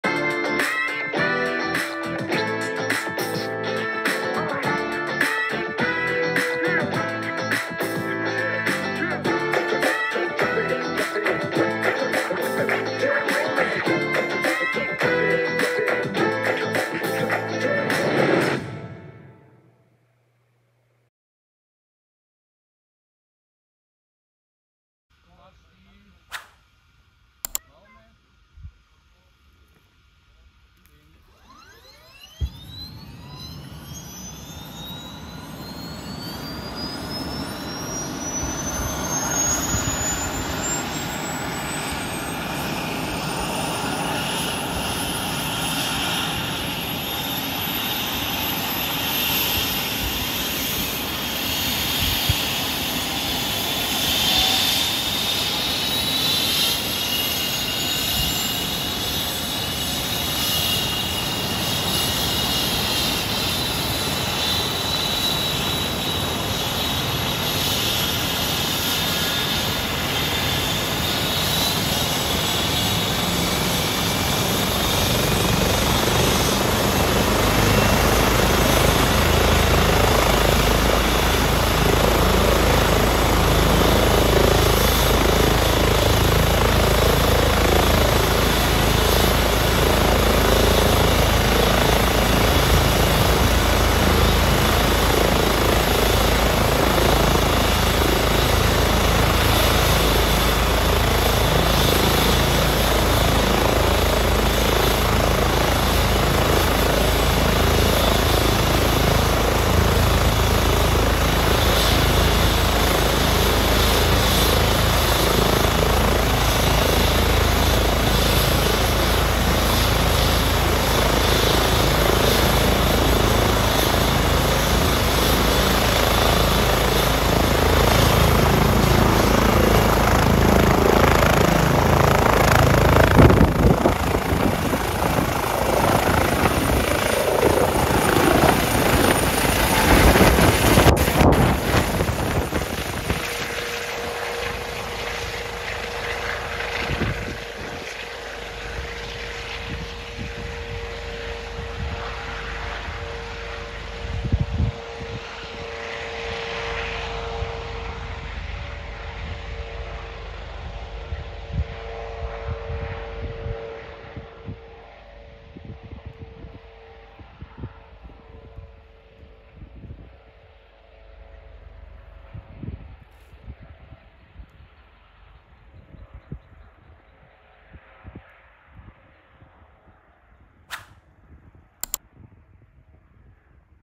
🚒🚁🚨[Christoph41 startet spektakulär in Weissach]🚨🚁🚒
Heute am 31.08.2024 ist der Rettungshubschrauber Christoph41 um circa 10:50Uhr in Weissach gestartet und flog wieder an das Krankenhaus Leonberg der Notarzt ist im RTW mitgefahren.

Jetzt unterschreiben damit Christoph 41 vielleicht doch in Leonberg bleibt.
Lasst ein Abo und ein Like da!